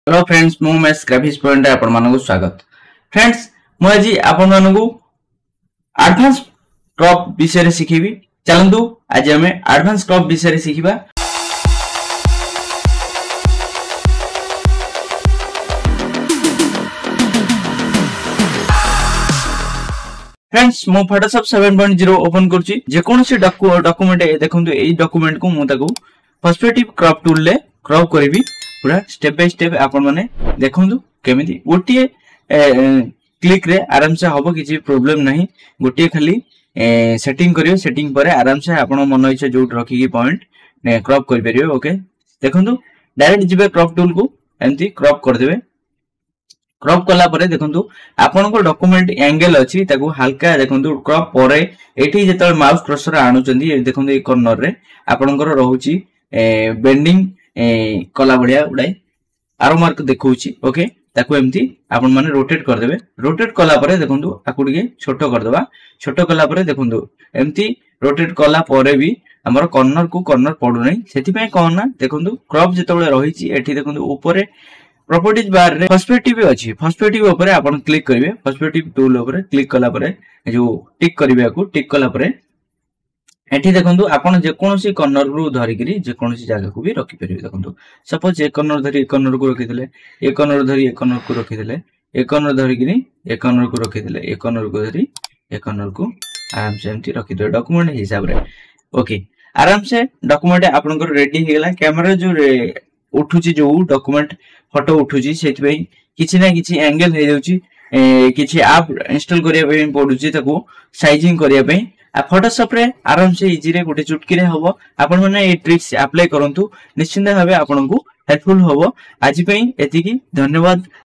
How to Use Prespective Crop Tool in Photoshop 7.0
In this video I will share you how to use Prespective Crop Tool in Photoshop 7.0